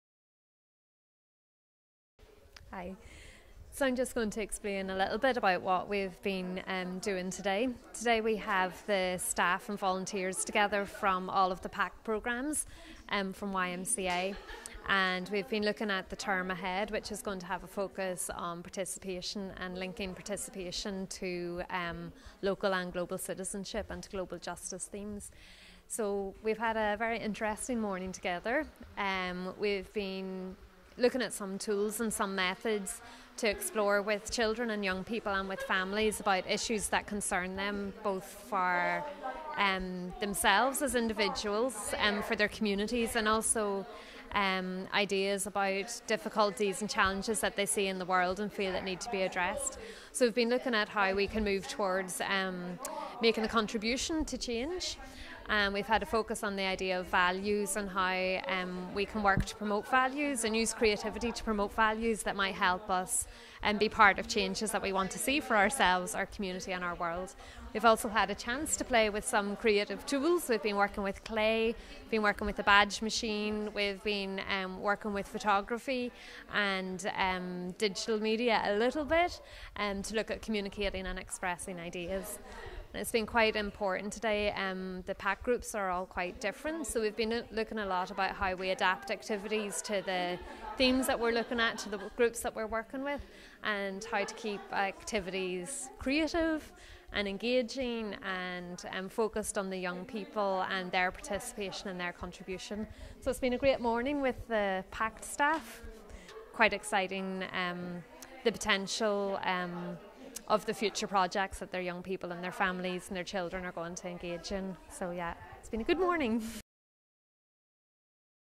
Jessica Carson - YMCA Ireland PAKT training day 7 January 2014
Jessica outlines the training programme for the day